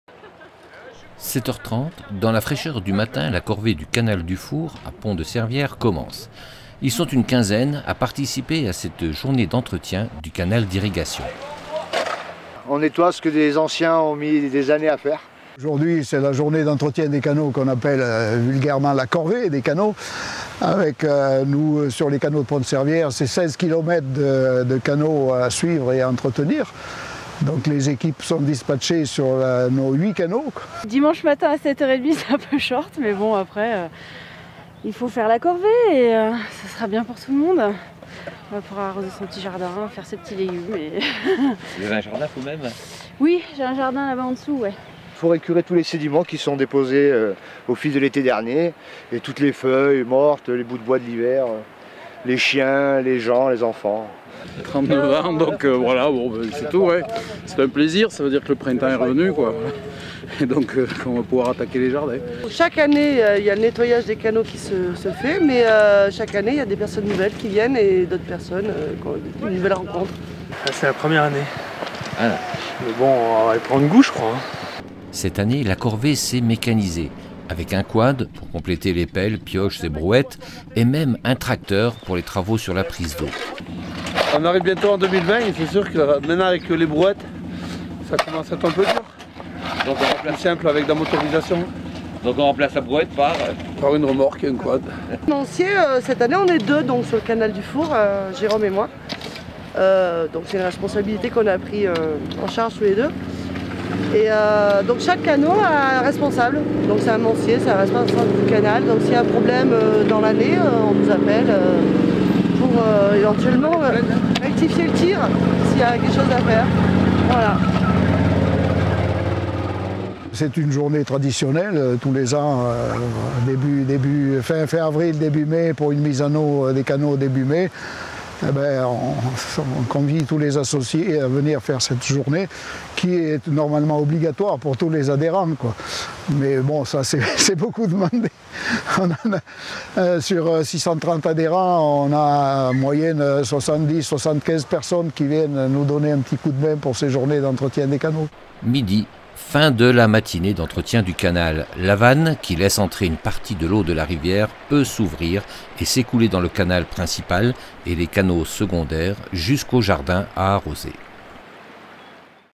Nettoyage de printemps pour les canaux de Pont-de-Cervières
En Briançonnais, les canaux d’arrosage qui date, pour la plupart, du moyen-âge, sont toujours utilisés. Même si les activités agricoles sont plus modestes qu’autrefois, les jardins ont toujours besoin d’eau.
A chaque printemps revient la journée d’entretien de ces canaux nécessaire avant la remise en eau. C’est la fameuse « corvée ». Une corvée très conviviale, ce dimanche matin le long du canal du Four, à Pont-de-Cervières.
Reportage Michel Toupet pour D!CI TV.
Avril 2019.